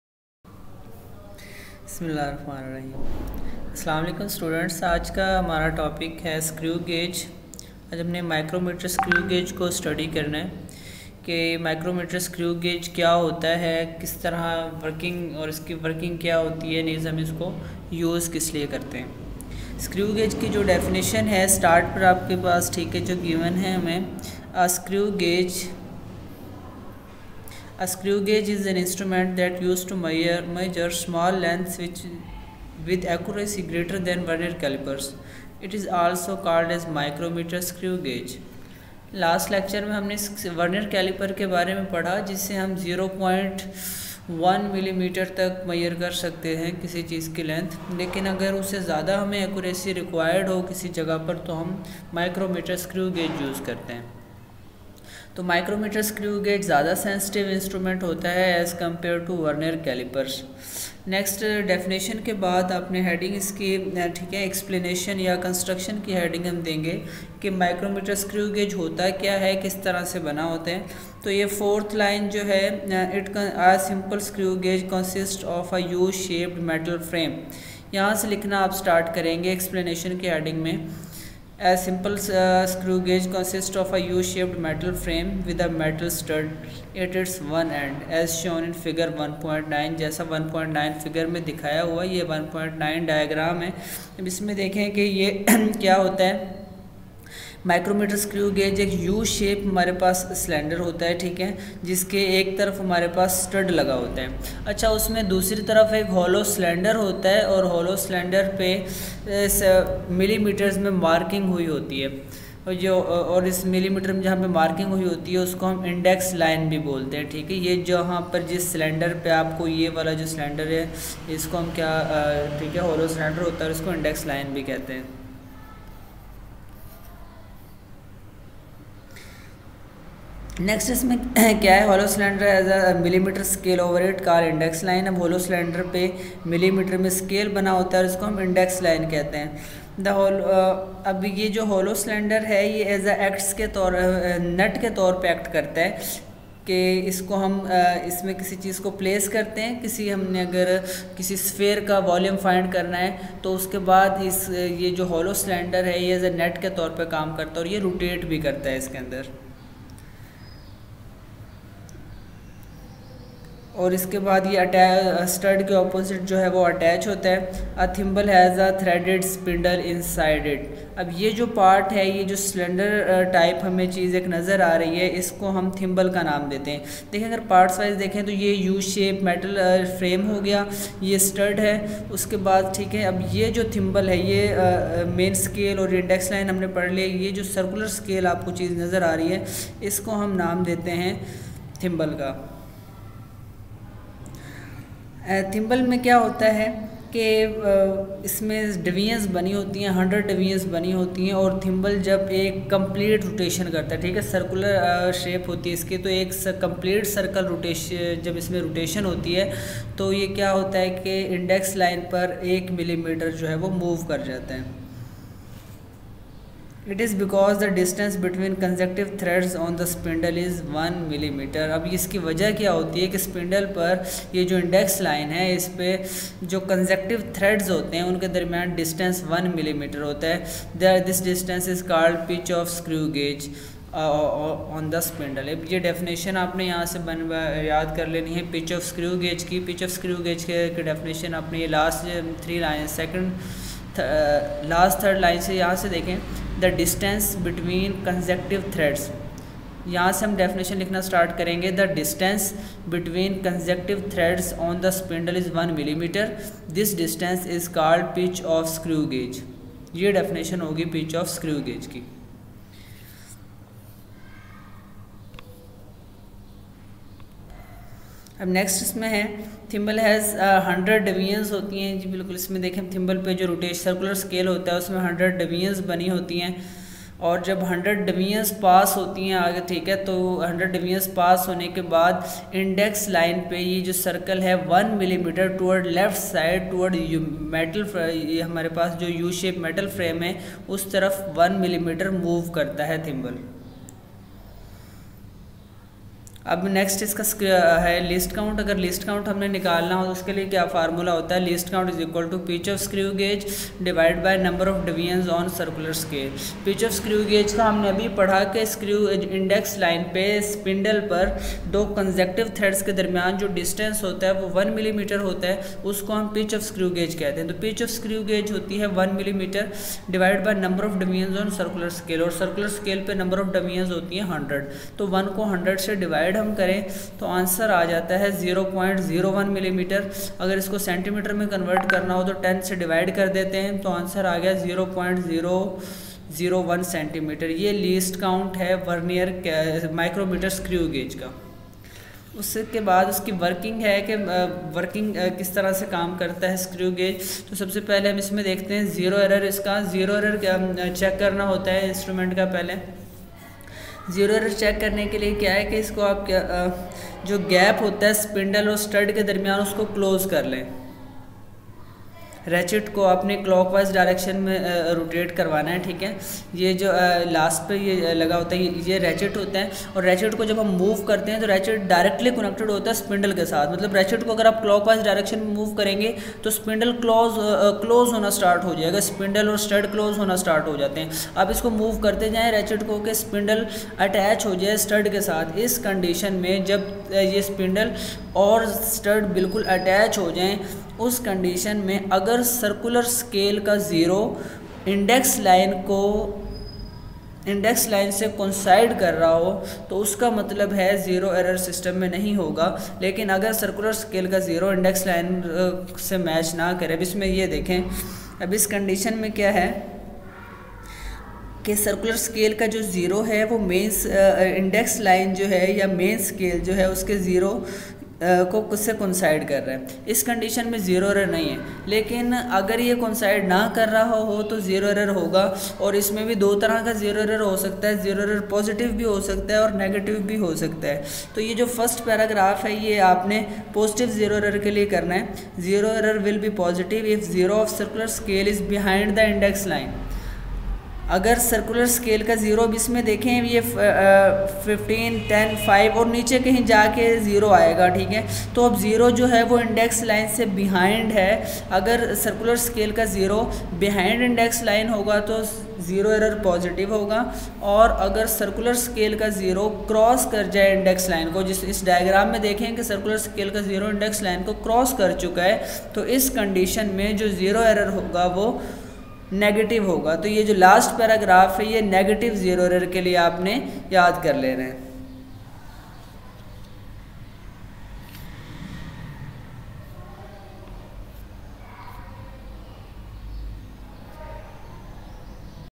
Screw Gauge Chapter-01/9th Physics
This vedio covers following questions
1.what is pitch.least count of S,G
2.zero error of screw gauge
3. possitive & negative zero error